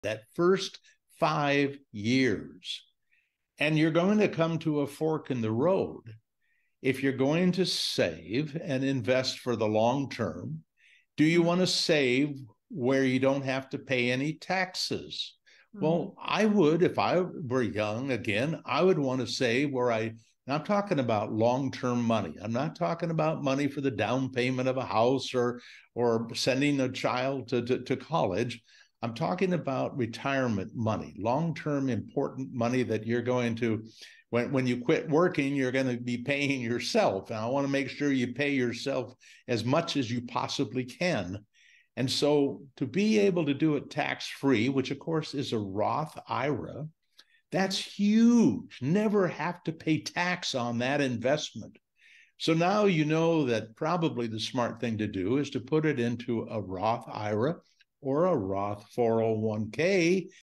Maximize Your Retirement Savings with a Tax-Free Roth IRA
Looking to save and invest for the long term? Learn how to grow your retirement money tax-free by utilizing a Roth IRA. Discover the benefits of this investment strategy and secure a comfortable future for yourself.